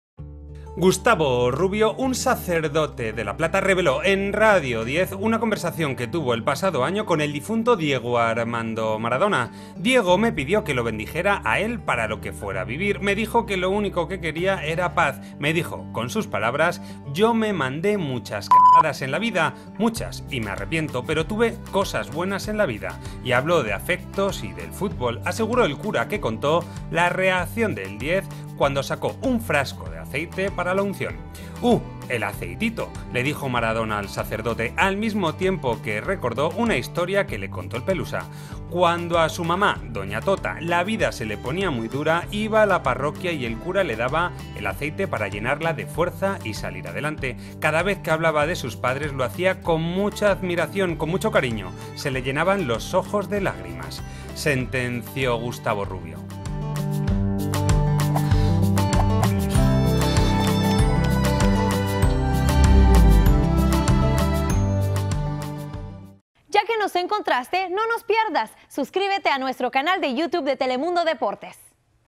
Un sacerdote desvela las últimas confesiones de Maradona | Telemundo Deportes
Video oficial de Telemundo Deportes. Gustavo Rubio, un sacerdote de La Plata, reveló una conversación que tuvo con Maradona y lo que le pidió.